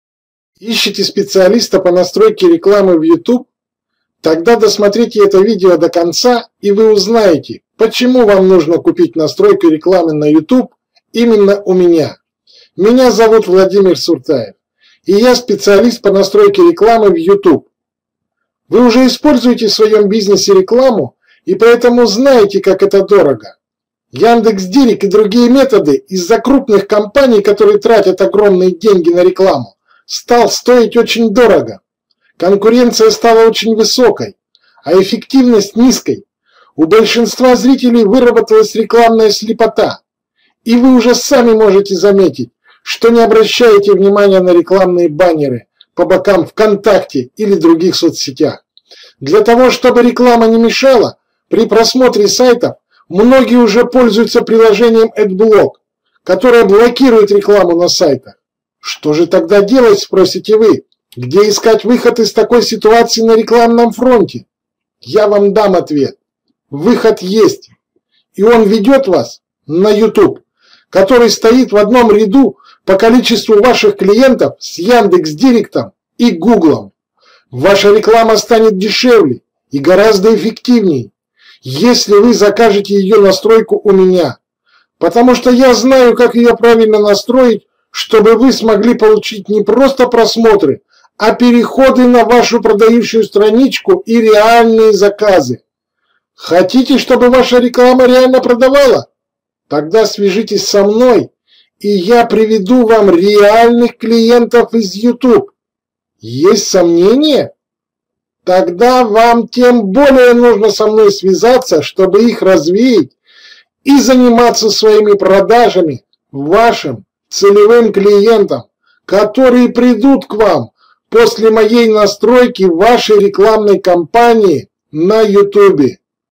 Настройка рекламы на YouTube
Ищете специалиста по настройке рекламы в Ютуб? Тогда Досмотрите это видео до конца, и вы узнаете почему вам нужно купить настройку рекламы на ютуб у меня!
Меня зовут Владимир Суртаев и я специалист  по настройке рекламы в YouTube.
вы уже используете в своем бизнесе рекламу и поэтому знаете как это дорого, яндекс директ и другие методы из за крупных компаний, которые тратят огромные деньги на рекламу, стал стоить очень дорого. Конкуренция стала очень высокой , а эффективность низкой, у большинства зрителей выработалась рекламная слепота и вы уже сами можете заметить, что не обращаете внимание на рекламные банеры по бокам в контакте или других соцсетях, Для того что бы реклама не мешала при просмотре сайтов, многие пользуются приложением AdBlock , которое блокирует рекламу на сайтах.
Что же тогда делать спросите вы? Где искать выход из такой ситуации на рекламном фронте?
Я вам дам ответ, Выход есть и он ведет вас на Ютуб, который стоит в одном ряду по количеству ваших клиентов с Яндекс директом и Гуглом! Ваша реклама станет дешевле и гораздо эффективней, если вы закажете ее настройку у меня, потому что я знаю, как ее правильно настроить что бы смогли получить не просто просмотры, а переходы на вашу продающую страничку и реальные заказы!
Хотите что бы ваша реклама реально продавала? Тогда свяжитесь со мной и я приведу вам реальных клиентов из Ютуб!
Есть сомнения? Тогда вам тем более нужно со мной связаться что бы их развеять и заниматься своими продажами вашим целевым клиентам, которые придут к вам, после моей настройки, вашей рекламной компании на Ютубе.